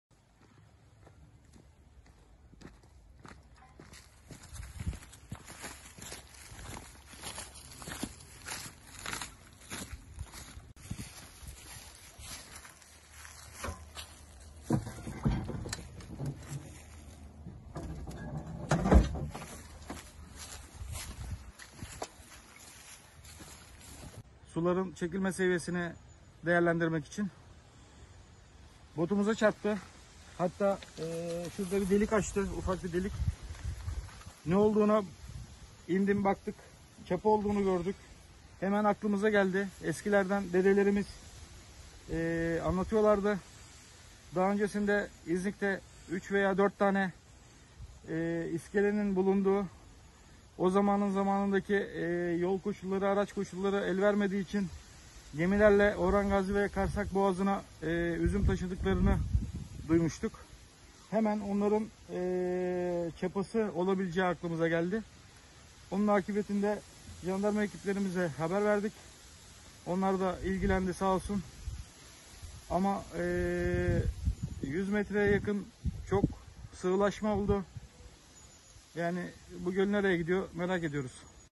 SULAR ÇEKİLDİ İZNİK GÖLÜ’NDE TARİHİ ÇAPA BULUNDU
İznik Gölü'nde su çekildi, tarihi eser olduğu sanılan çapa bulundu